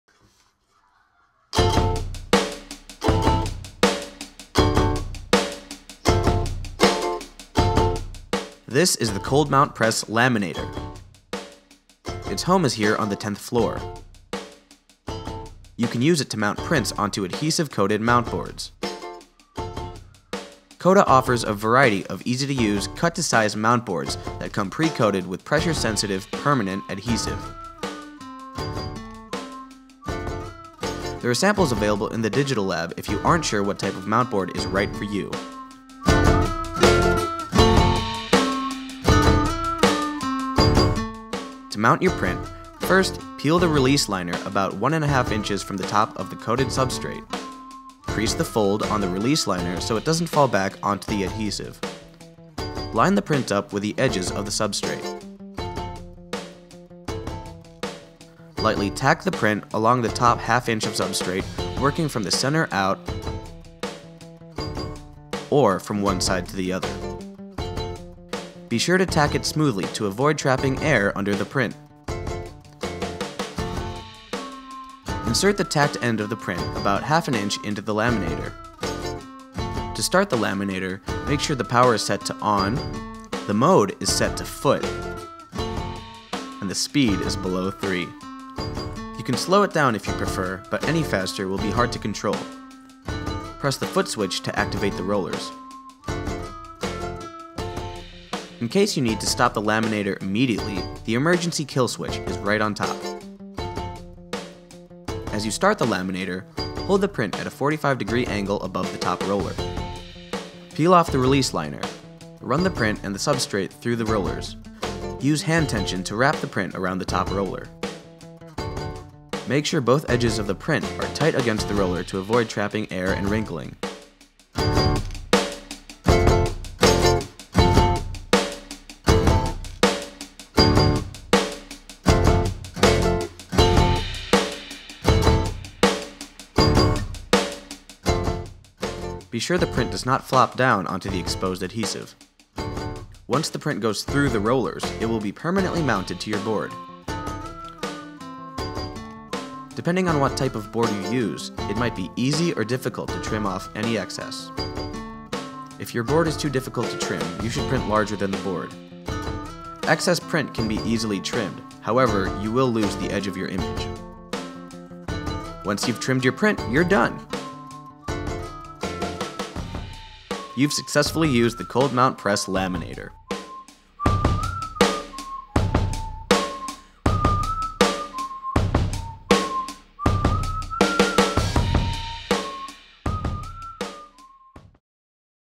Cold Mount Press Laminator
A demonstration video on how to use the CODA Cold Mount Press Laminator in the 10th Floor Darkrooms of the Columbia College Chicago Photography Department.

Video, Narration, and Music by Rudy Lorejo.

Edit: I am not a dealer or in anyway involved in the sale and distribution of this product so please stop asking me for my phone number. If you have any questions about this product well good luck because I do not have any answers.